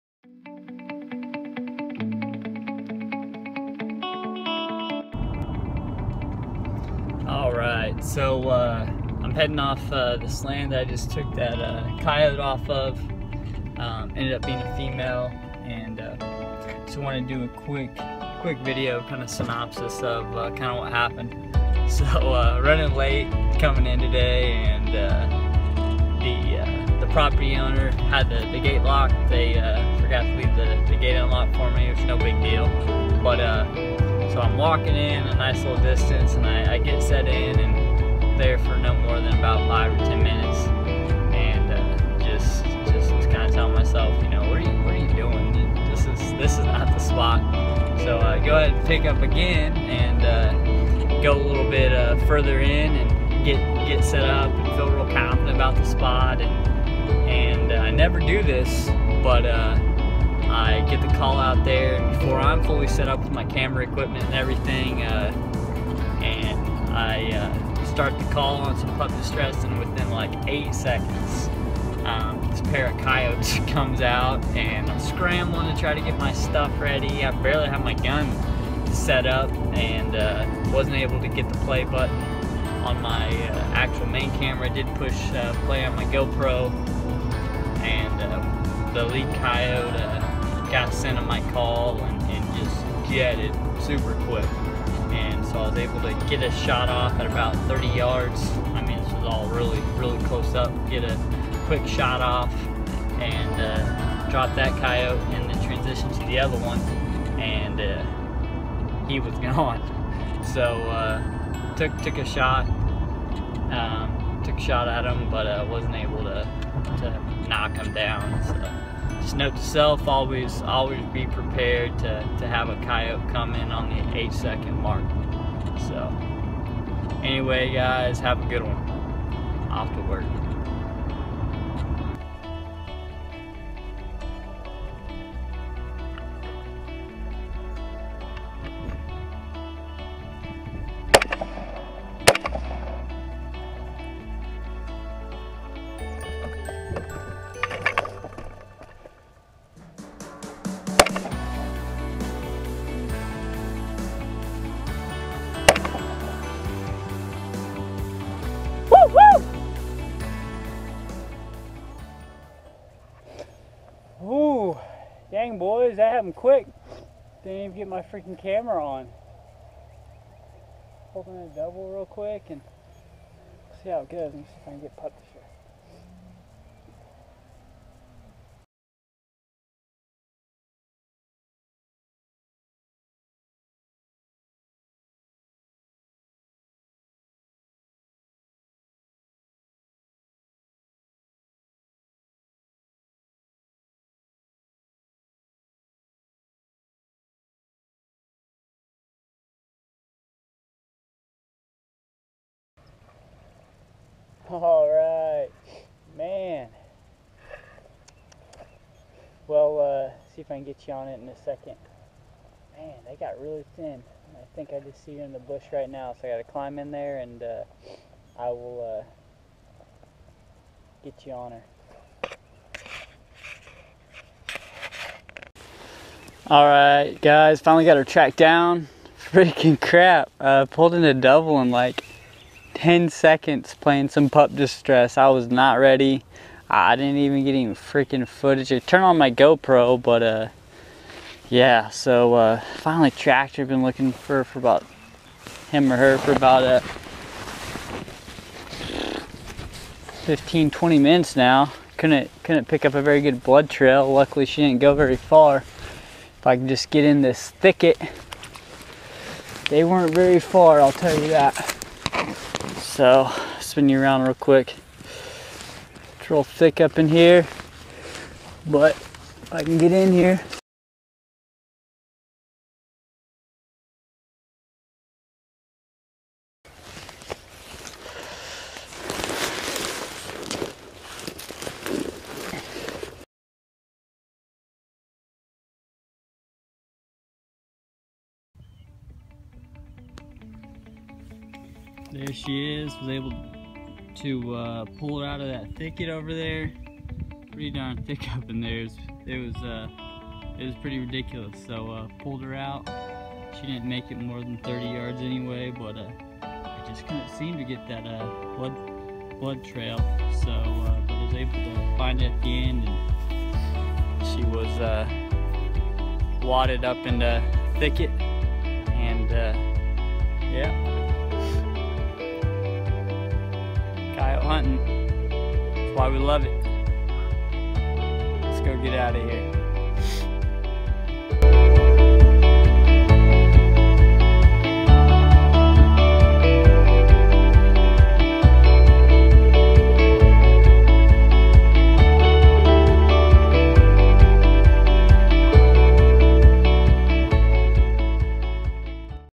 How to call two coyotes in 8 seconds
Went out for a morning coyote hunt and was able to call in a double in 8 seconds. The end result was one coyote down.